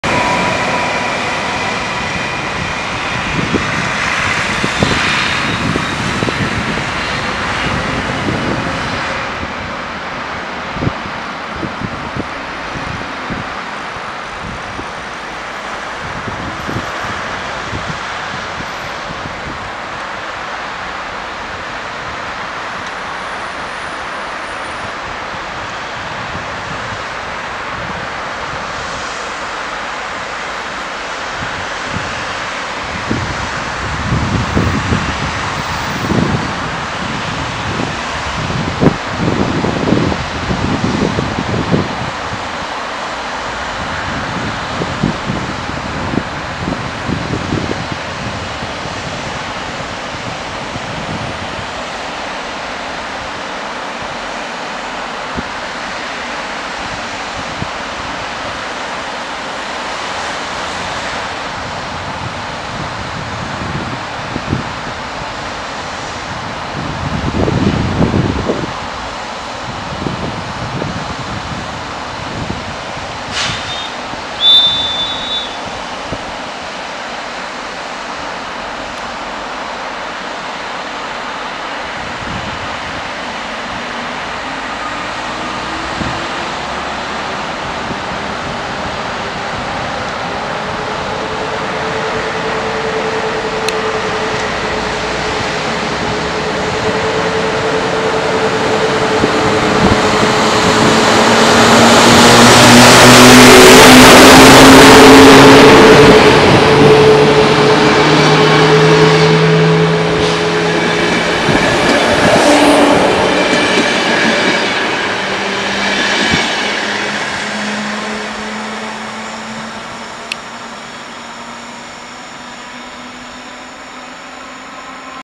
BRZESKO OKOCIM - IC104 PORTA MORAVICA GRAZ - PRZEMYŚL
BRZESKO OKOCIM - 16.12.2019 godz.16:30, opóźniony 10 min
IC104 PORTA MORAVICA Graz Hbf - Przemyśl Gł
Prowadzi lokomotywa EP09-046 z wagonami Koleji Austriackich "OBB"